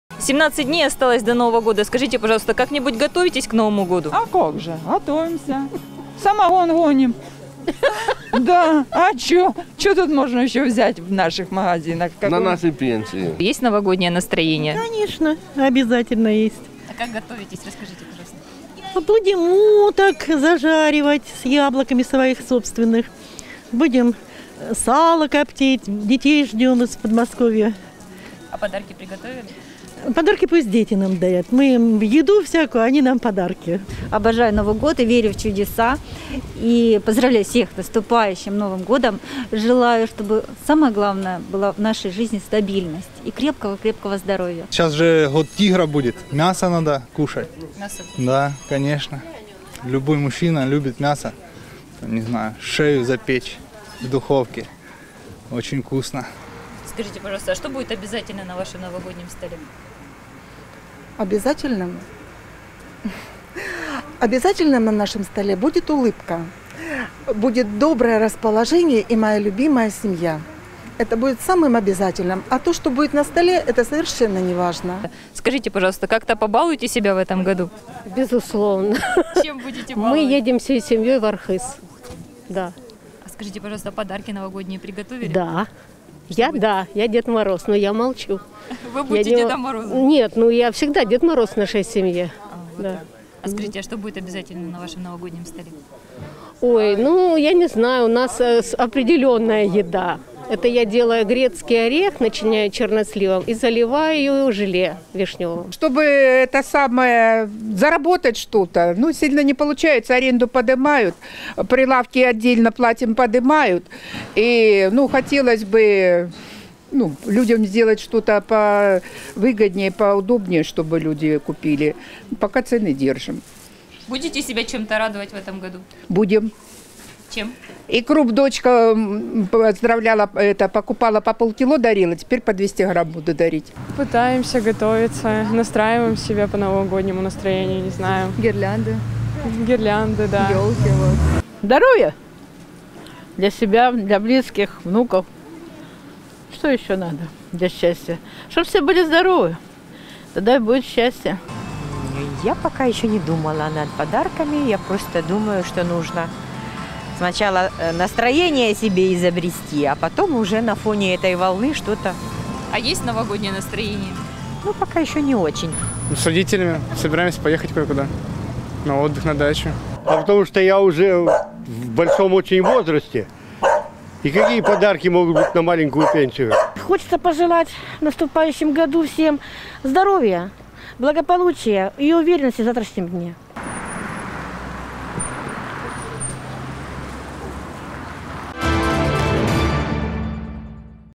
Новогоднее настроение
До Нового года осталось чуть больше 2 недель. Однако по данным  аналитического ресурса «Чек Индекс» «Новогодний набор», куда входят искусственная ель, гирлянды, елочные шары, звезда, мишура, бутылка шампанского и мандарины, в ноябре 2021 года уже подорожал на 19% — до 4 тысяч девятисот тридцати пяти  рублей.  При этом стоимость одного килограмма мандаринов в Азове  составляет в среднем 120 руб., что  на четверть больше, чем год назад. Что думают азовчане о приближении самого волшебного праздника в году, и готовы ли они к нему морально и финансово, поинтересовались наши корреспонденты.